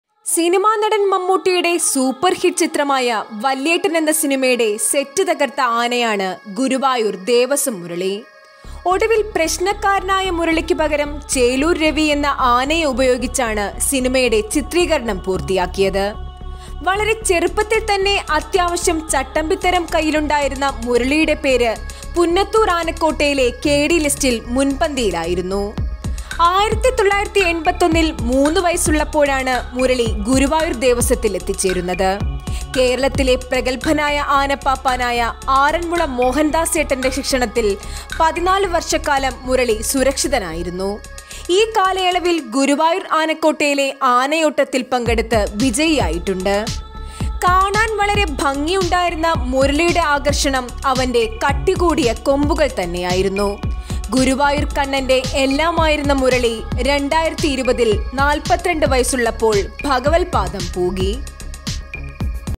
സിനിമാ ഷൂട്ടിങ് സെറ്റ് തകർത്ത ആനയെ മര്യാദക്കാരനാക്കിയ പാപ്പാൻ | guruvayoor devaswom murali elephant
സിനിമാ ഷൂട്ടിങ് സെറ്റ് തകർത്ത ആനയെ മര്യാദക്കാരനാക്കിയ പാപ്പാൻ | Aanakathakal | Guruvayoor elephant | guruvayoor devaswom murali elephant